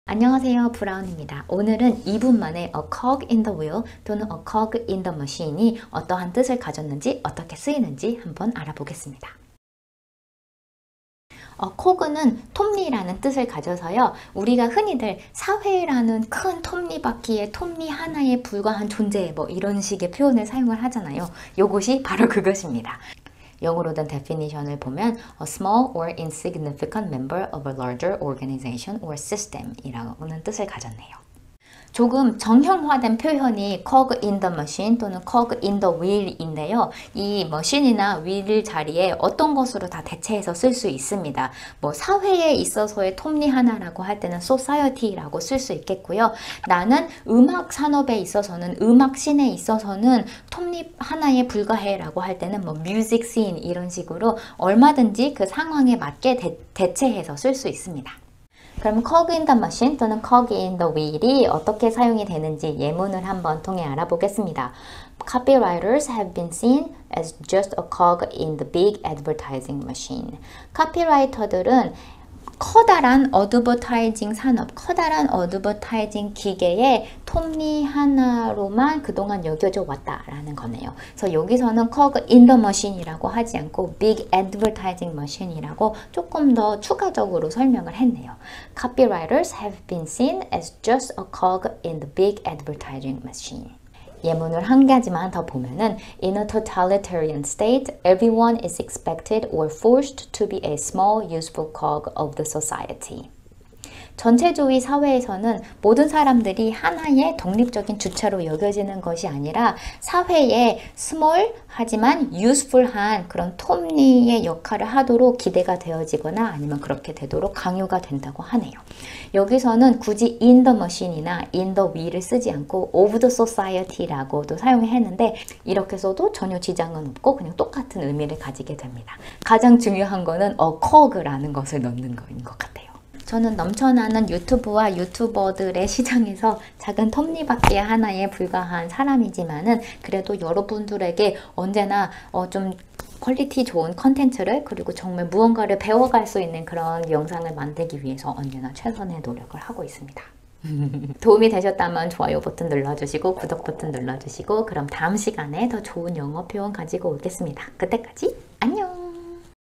2분만에 배우는 영어 I A Cog in the Machine | 영어공부 | 고급영어 | 원어민 영어
안녕하세요! 브라운영어입니다.

이번엔 금방 돌아온  2분만에 배우는 영어표현 시리즈입니다.
이번 편에서는 A Cog in the machine / A cog in the wheel 에 대해서 알아보았습니다 :)
짧고 굵게 좋은 영어표현 배워가세용!